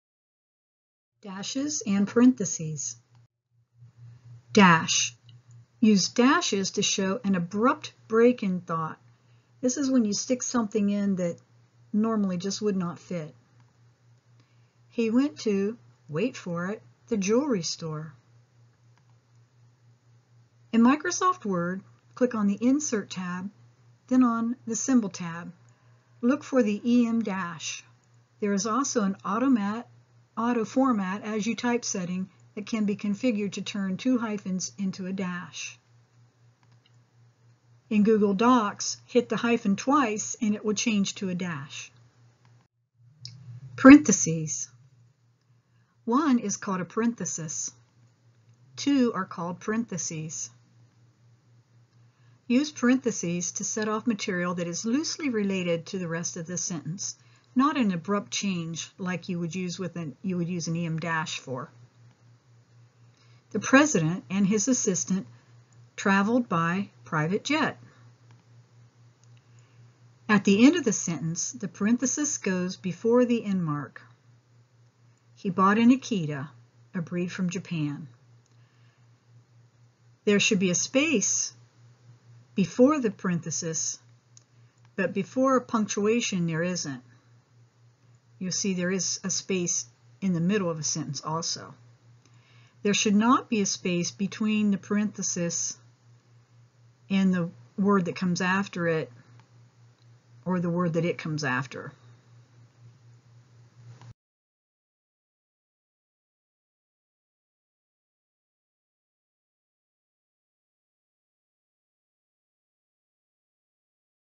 10.7 Dashes and Parentheses
This video is a supplement to Grammar Wired! It is geared toward 5th and 6th grades but can be used in all grades.